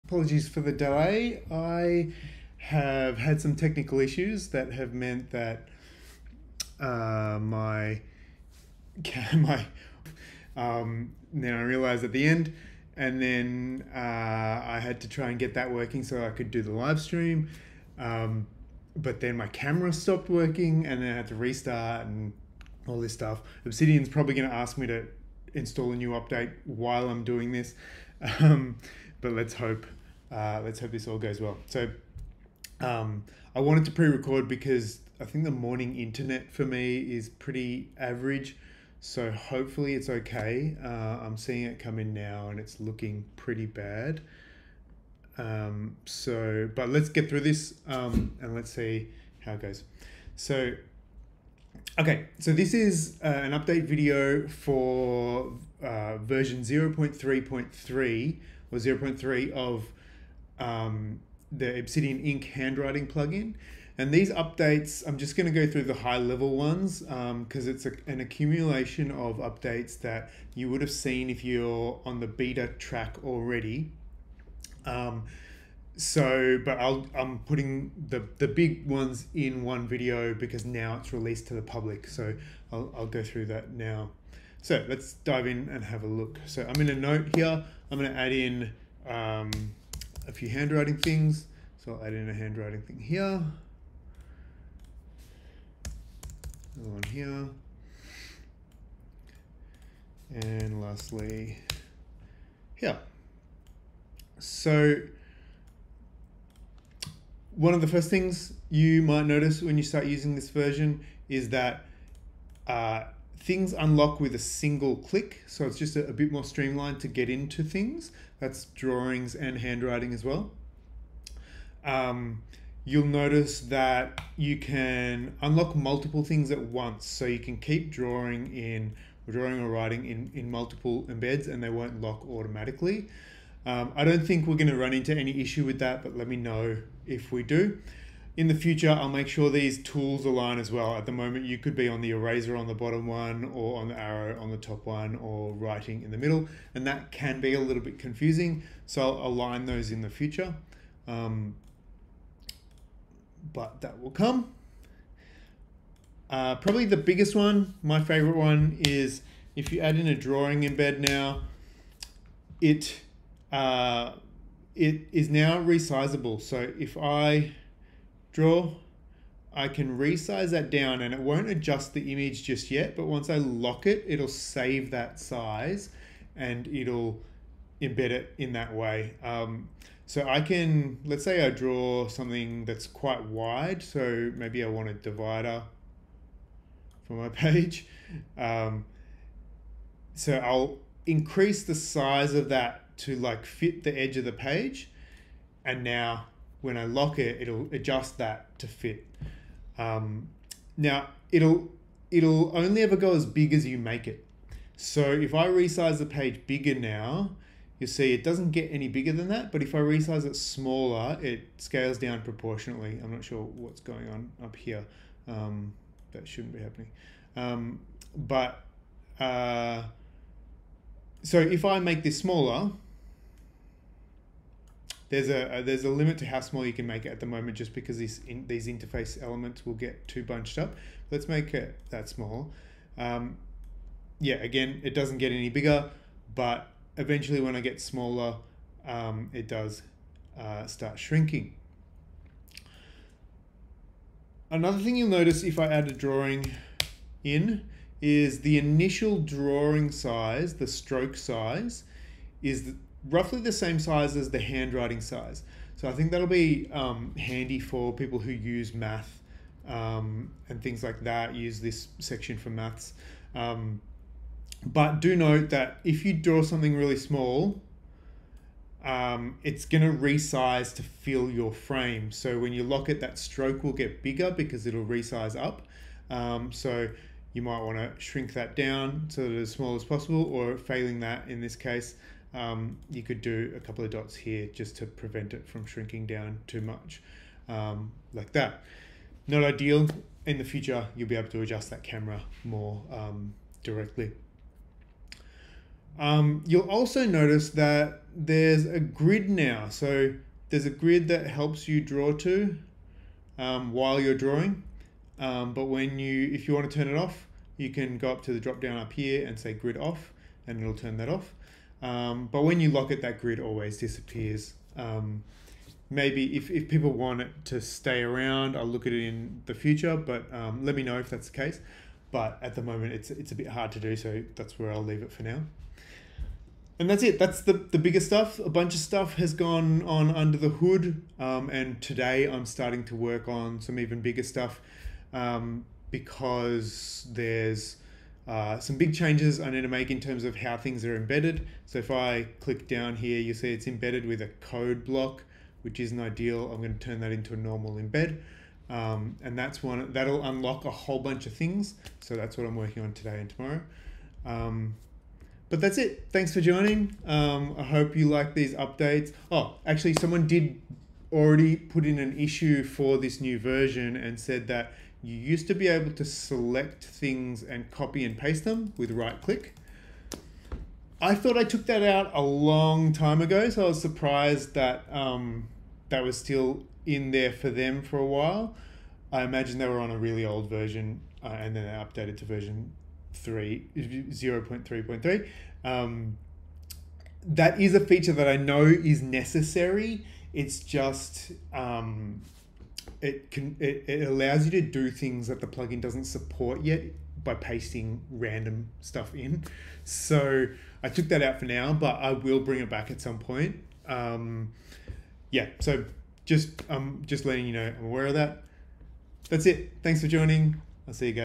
Obsidian Ink v0.3 (DesDevDiary)
Now that version 0.3 is released publicly, in this video I'll recap all the changes that have gone into it.
This release includes some useful updates to drawing (resizing, grid, etc.) as well as other more quality of life improvements.

Find the Ink plugin in Obisdian Community Plugins by searching "Handwriting".